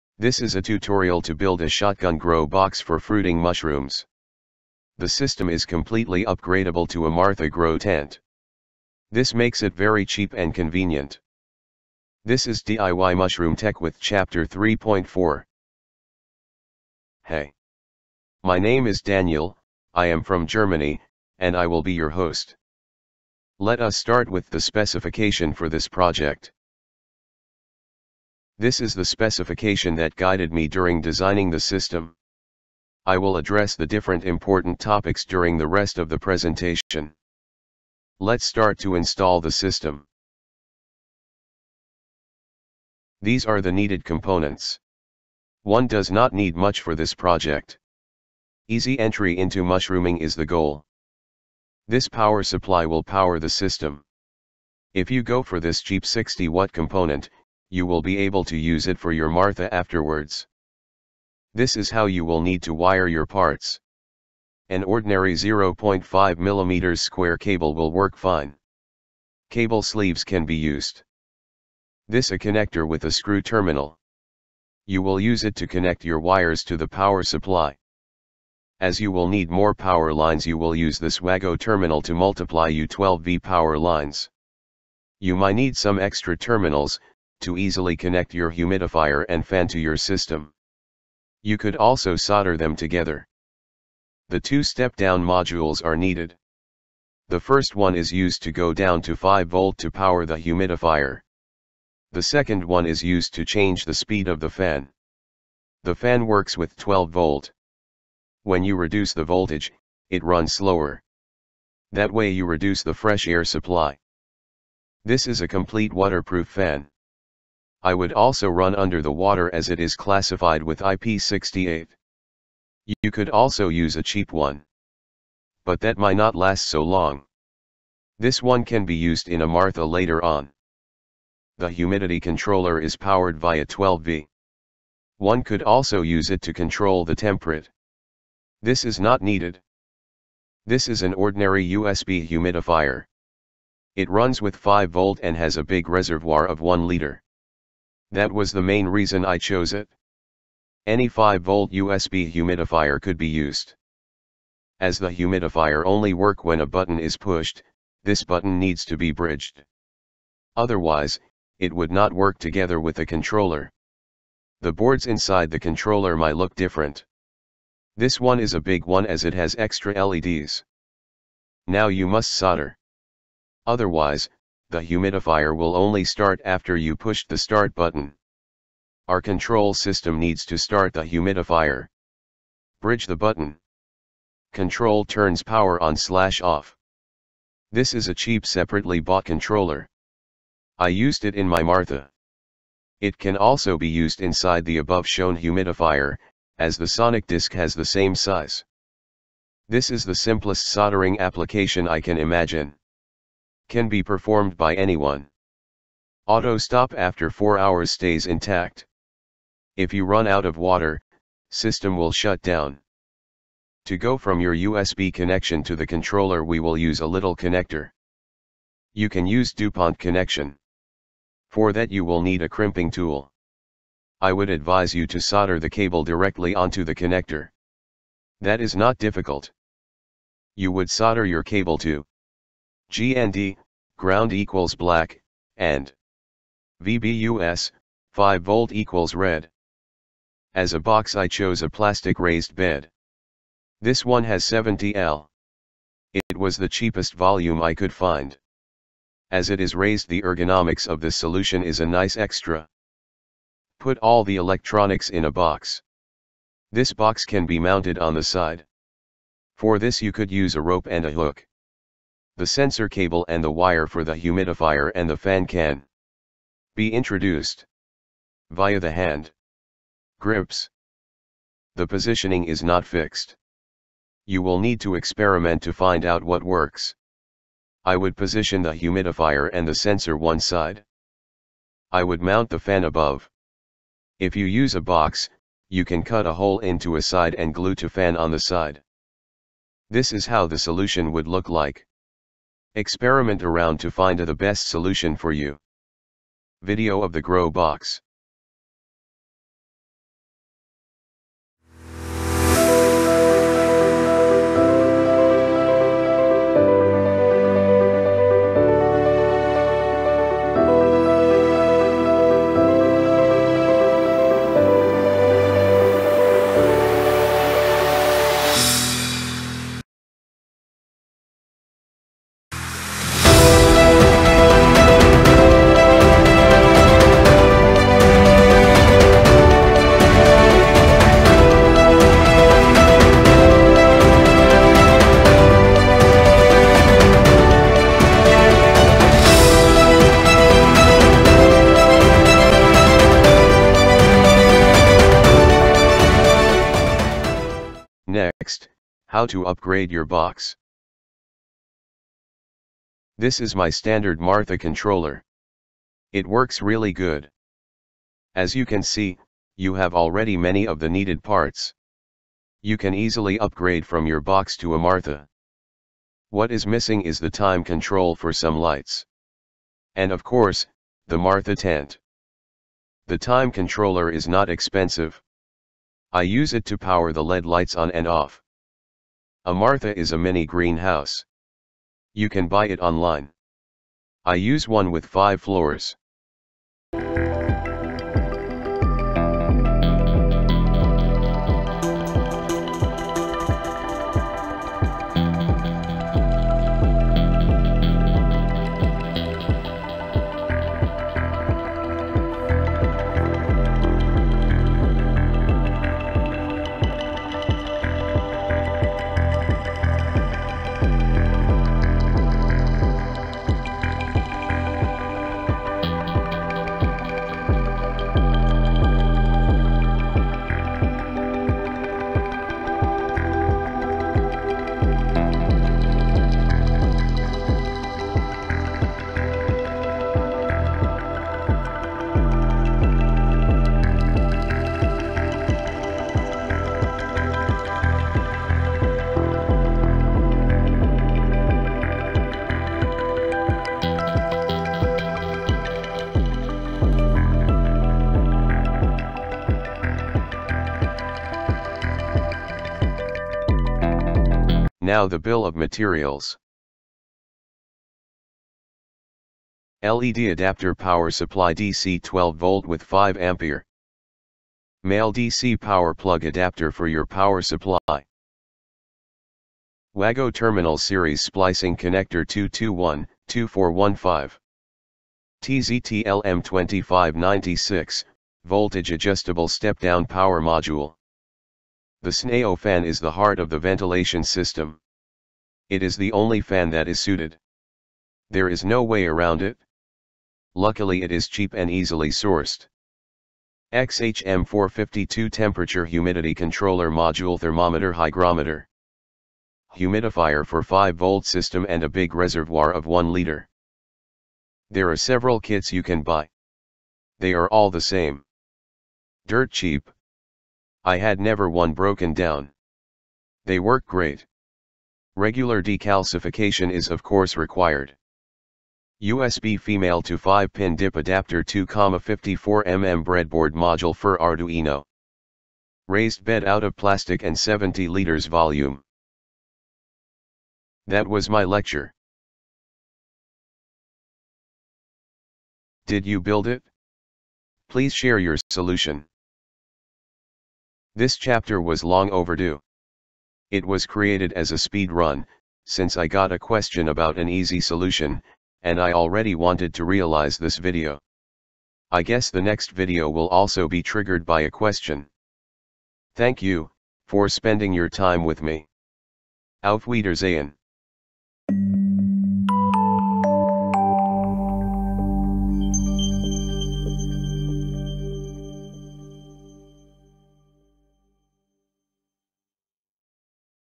3.4 Controlled Monotub Shotgun Fruiting Chamber
⚙️➡️🍄 In this video I will teach you how to build your shotgun fruiting environment. We will use a big plastic box for this. I used a raised bad, as it was cheaper and offers good ergonomics. Then we add a waterproof fan and a humidifier to it. We can control it with simple raspberry or Arduino components. All the parts can later be used, if one wants to build a complete martha. In that way no money is lost and only experience is gained!

🚀 For this build 3D printing is not needed and the technical requirements are reduced to the bare minimum. No programming is needed. This allows everyone a smooth and simple entry into the world of mushroom growing!

Update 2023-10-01: Please use the newer and more advanced XY-TR01 humidity controller with the SHT20 Sensor instead of the older XH-M452 with the AM2120 Sensor.

🌟Happy entry into mushrooming!

🕘✅🍄 Jump marks:
◼ Introduction: 0:00
◼ Specifications: 0:29
◼ Installation: 0:44
◼ Video Grow Box: 5:00
◼ Upgrade to Martha: 5:51
◼ Bill of Materials: 7:35
◼ Closure: 8:57

Main Controlling Elements:
◼LM2596 2v-37V 3A Voltage Step Down converter
◼CN101A 12vdc digital timer
◼XH-M452 Temperature and Humidity controller
◼Sanyo 9WP0812H401 imported Japanese IP68 waterproof fan 8025 12V cooling fan